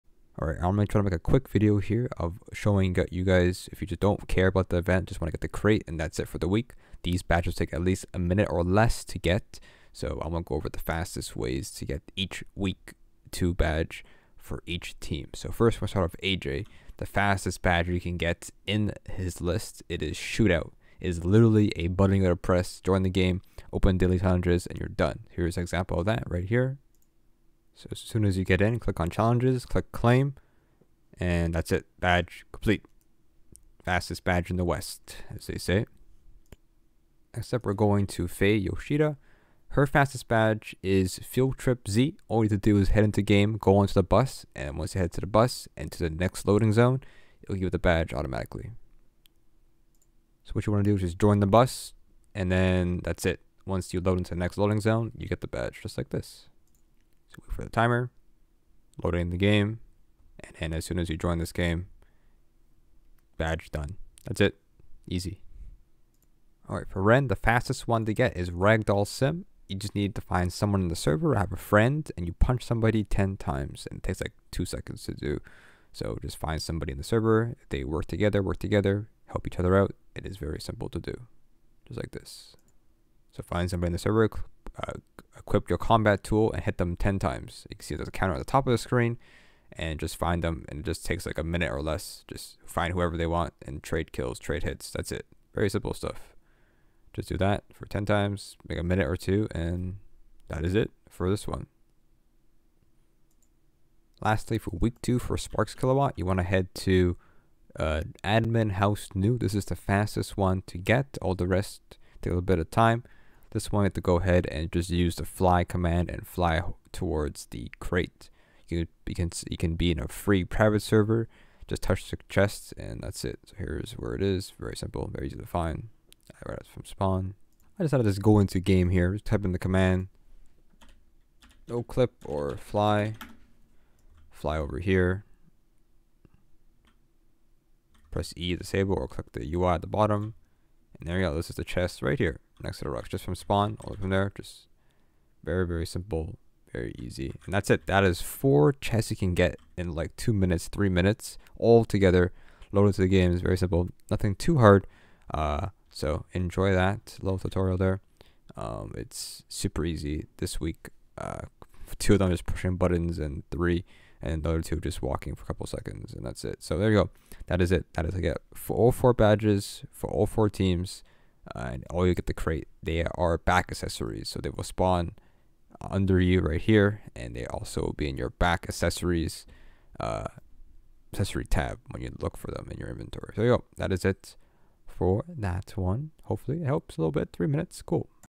Easiest Week 2 Crates for Each Team | Get them ALL Under 3 Minutes | FREE HATS | Metaverse Event
The 4 fastest badges you can earn from each team. Only if you don't care about the event and just want the crates for the week.